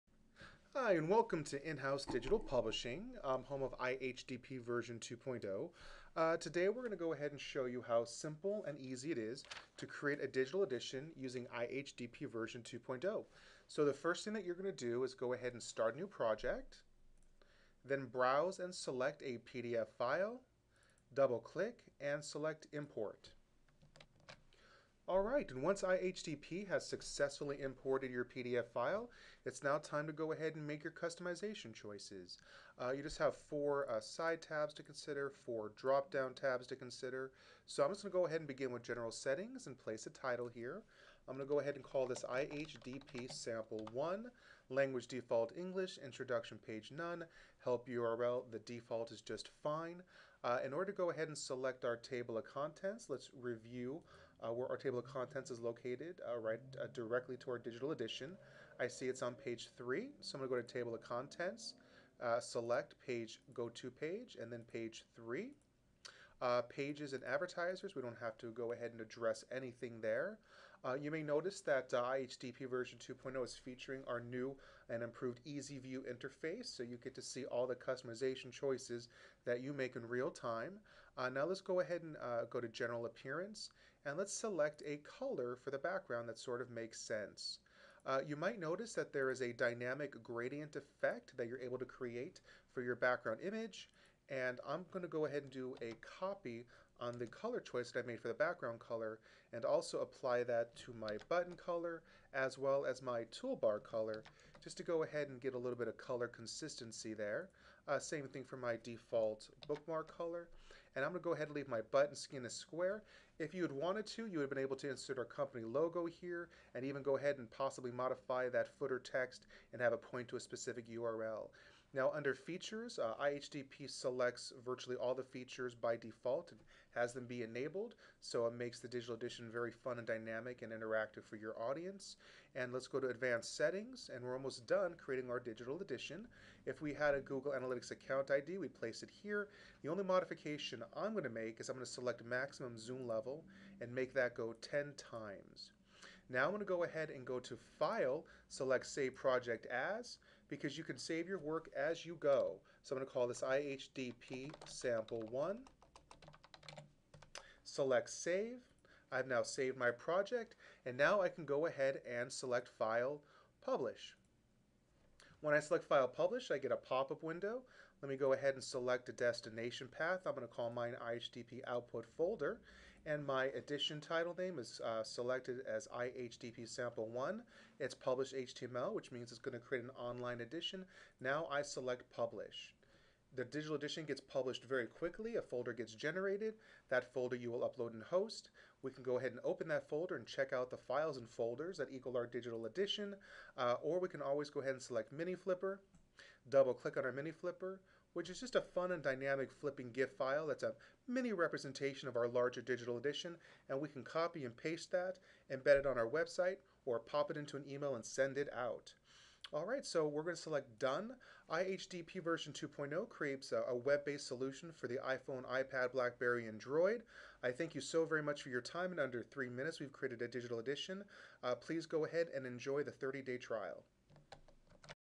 IHDP 2.0 Basic Intro Digital Page Flip Publication Software
In House Digital Publishing (IHDP) Software incorporates cutting edge technology into a user friendly point and click software application for the purposes of creating dynamic and interactive digital publications.

IHDP 2.0 Basic Introduction in creating exciting digital page flip publications!